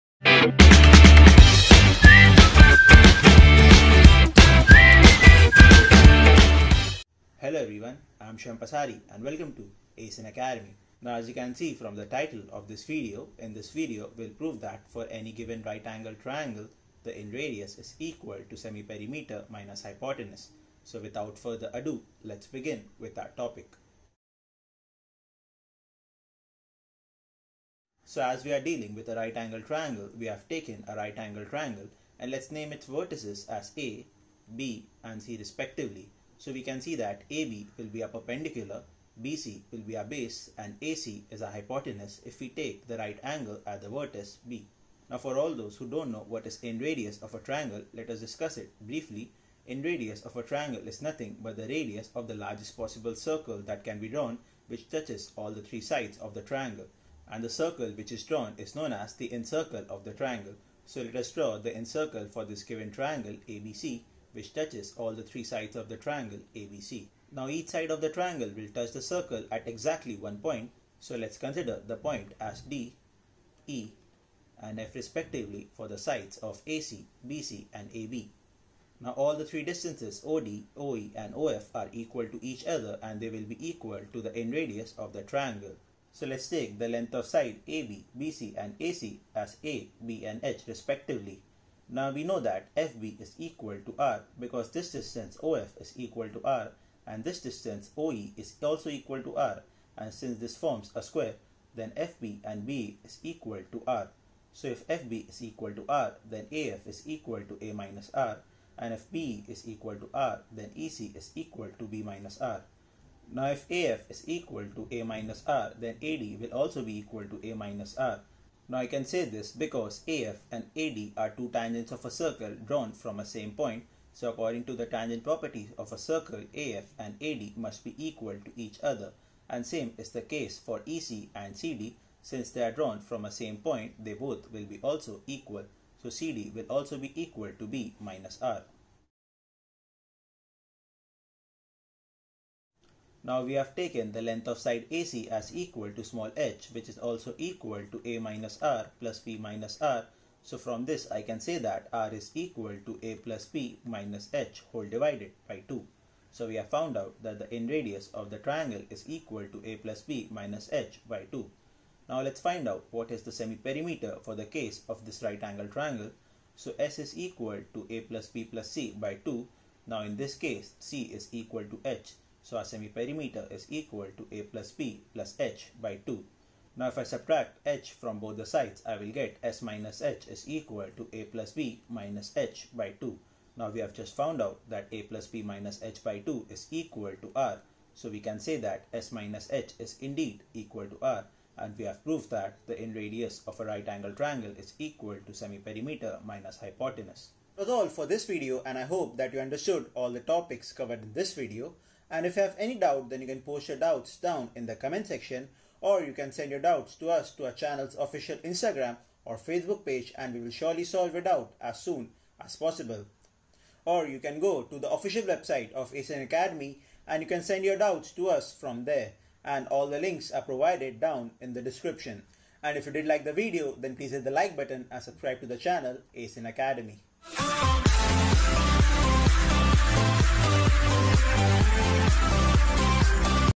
In-radius = Semi-perimeter - hypotenuse | Proof | [ GEOMETRY ]
In this video, we will prove that In-radius = Semi-perimeter - hypotenuse  , it is a very important formula in geometry and also one of the most useful ones.

Topics discussed in this video :

1. What is In-radius.
2. What is In-Circle.
3. What is a tangent.
4. how to prove that In-radius = Semi-perimeter - hypotenuse

PROOF

1. Take the length of sides as a,b, and h.
2. a =( a -r ) + r , b =( b - r ) + r , and h = ( a - r ) + ( b - r )
3. from value of h, r = (a + b - h)/2
4. s = ( a + b + h )/2
5. subtracting h from both the sides
6. s - h = r ( HENCE PROVED )